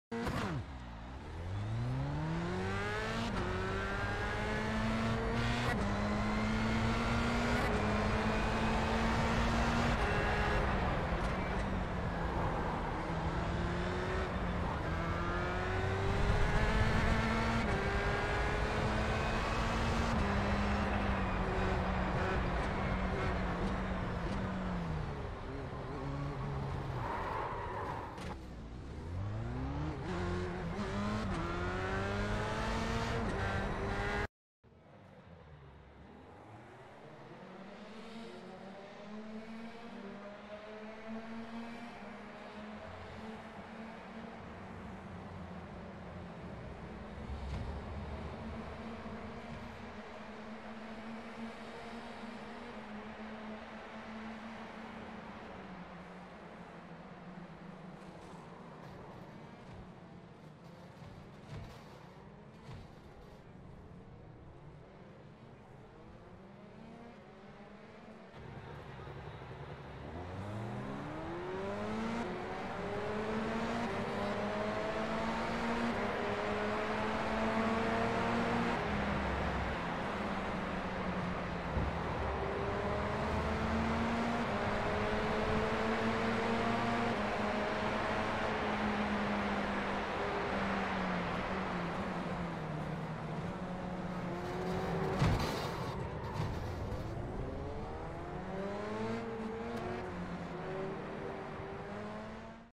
F3 Hockenheimring Race Start (21S3)
Had a sketchy start to my first race of the week, but it went well enough in the end.